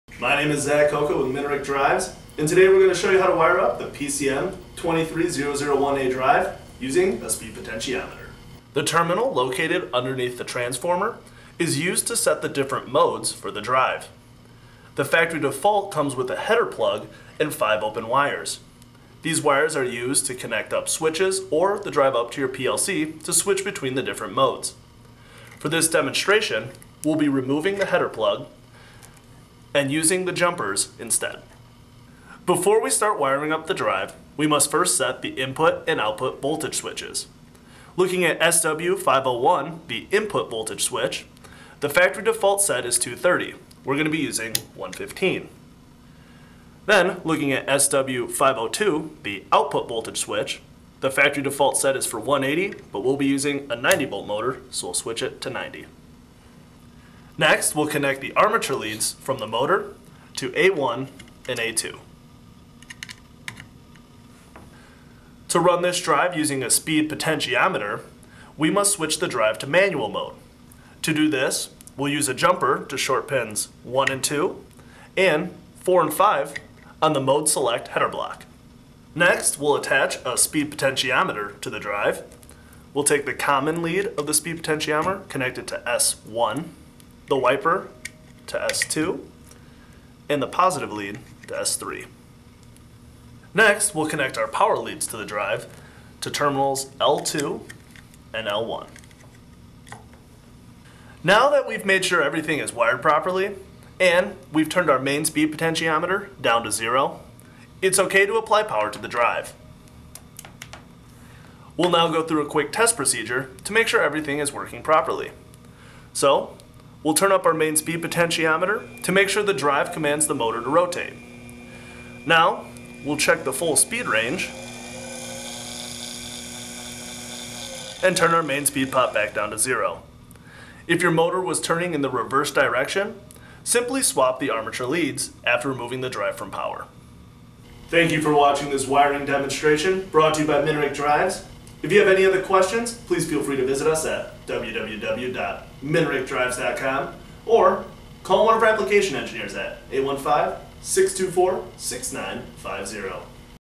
Minarik Drives - PCM23001A Wiring Speed Pot
This video shows how to wire up a potentiometer to a PCM23000 Series drive for either Manual or Ratio mode.